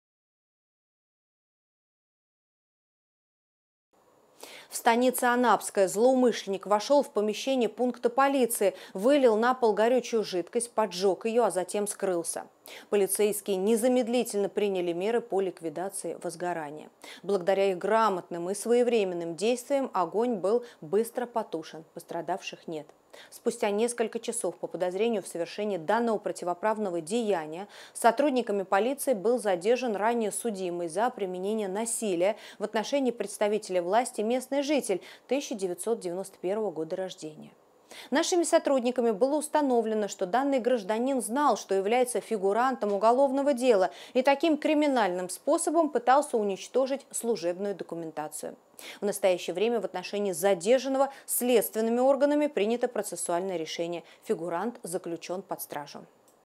Ирина Волк: В Краснодарском крае задержан подозреваемый в попытке поджога пункта полиции
«В станице Анапская злоумышленник вошел в помещение пункта полиции, вылил на пол горючую жидкость, поджег ее, а затем скрылся. Полицейские незамедлительно приняли меры по ликвидации возгорания. Благодаря их грамотным и своевременным действиям огонь был быстро потушен, пострадавших нет», - сообщила официальный представитель МВД России Ирина Волк.
Спустя несколько часов по подозрению в совершении данного противоправного деяния сотрудниками полиции был задержан ранее судимый за применение насилия в отношении представителя власти местный житель 1991 года рождения.

«Установлено, что данный гражданин знал, что является фигурантом уголовного дела и таким криминальным способом пытался уничтожить служебную документацию. В настоящее время в отношении задержанного следственными органами принято процессуальное решение. Фигурант заключен под стражу», - уточнила Ирина Волк.